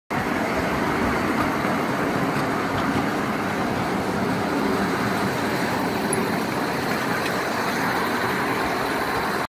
Miniature Garden Railway in Poland
A ride around a garden on a miniature train. Location: Nietoperek near Miedzyrzecz in western Poland